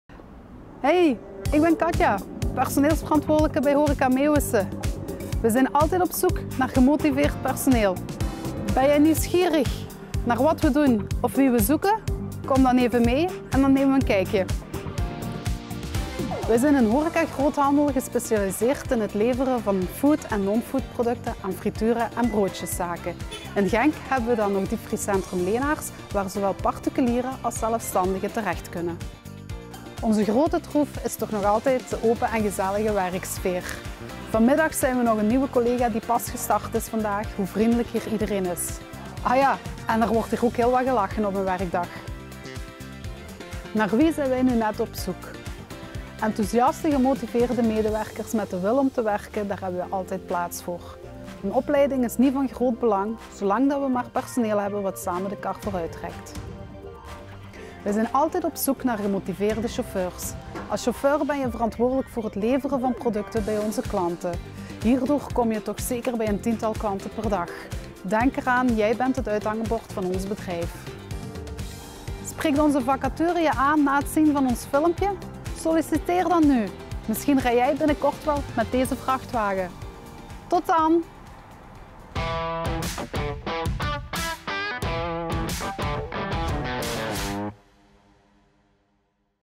Bij Horeca Meeuwissen trek je samen met je collega's de kar vooruit!
De grote troef van Horeca Meeuwissen? Dat is de open en gezellige werksfeer!